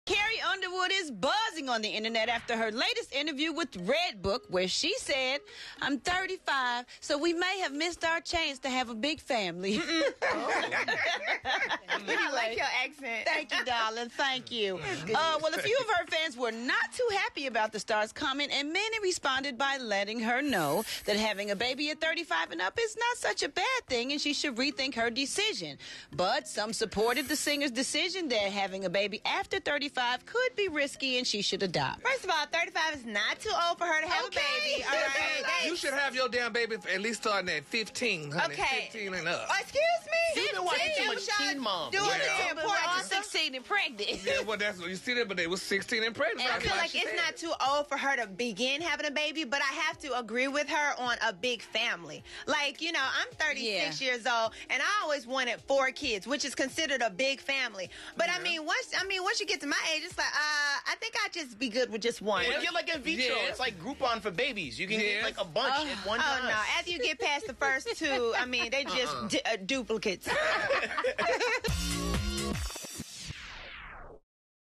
CARRIE UNDERWOOD SAYS SHE MISSED HER OPPORTUNITY TO HAVE A BIG FAMILY
Carrie Underwood says she can't "carrie" on her plans to have a big family, and her reasoning is dividing her fans. What's your take?